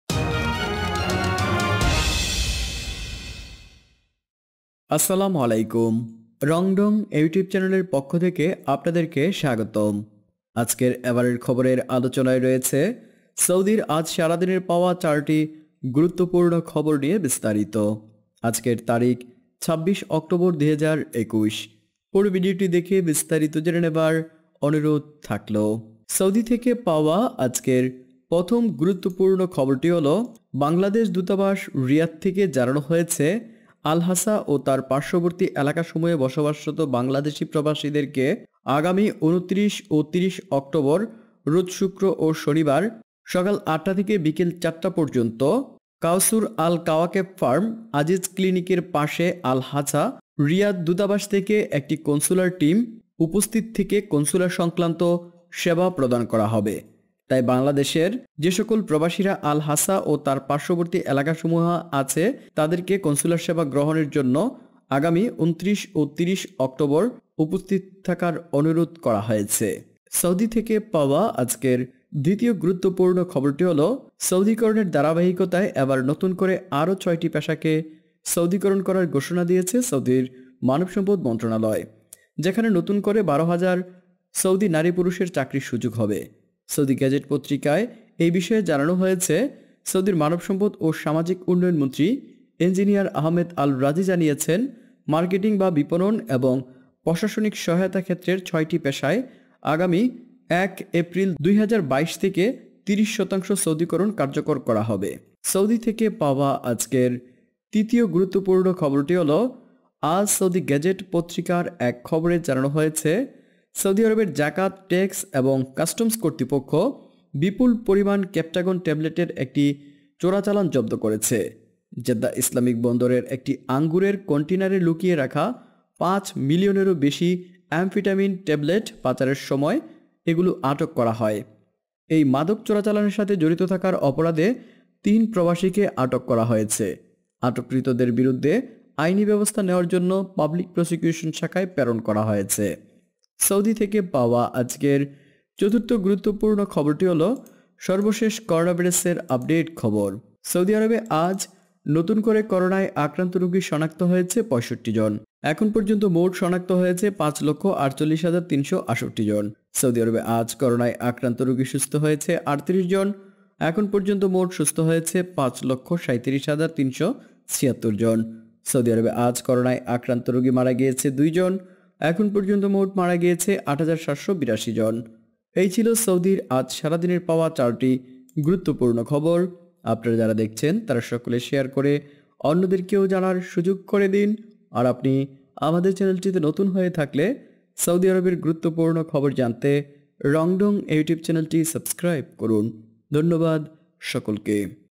সৌদির আজ সারাদিনের ৪ টি গুরুত্বপূর্ণ খবর | সৌদির ৪ টি নতুন খবর | তারিখ – ২৬/১০/২০২১
আজকের এবারের খবরের আলোচনায় রয়েছে  সৌদির আজ সারাদিনের ৪ টি গুরুত্বপূর্ণ খবর নিয়ে বিস্তারিত। তারিখ – ২৬/১০/২০২১। পুরো ভিডিওটি দেখে বিস্তারিত জেনে নেবার  অনুরোধ রইলো।